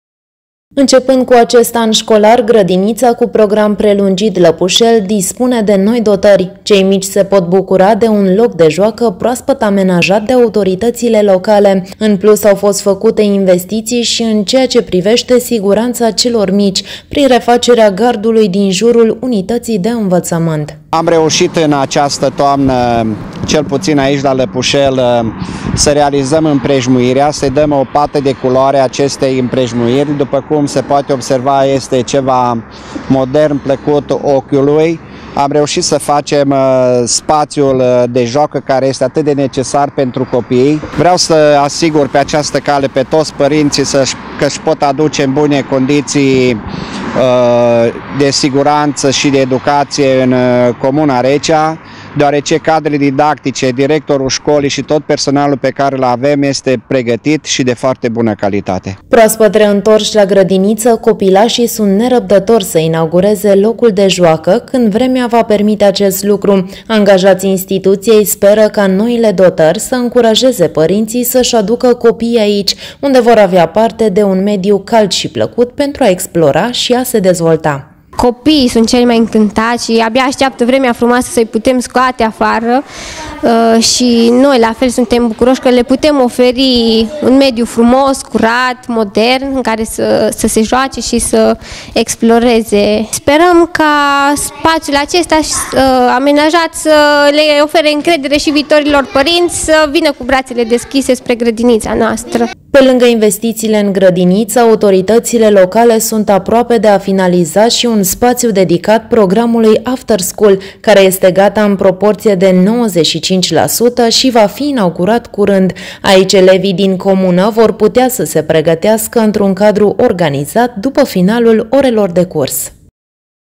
LOC DE JOACĂ NOU, AMENAJAT LA GRĂDINIȚA DIN LĂPUȘEL
Preșcolarii care frecventează Grădinița din Lăpușel se bucură din acest an de un nou loc de joacă. Investiția a fost realizată de autoritățile locale, care doresc să încurajeze părinții să-și aducă aici copiii.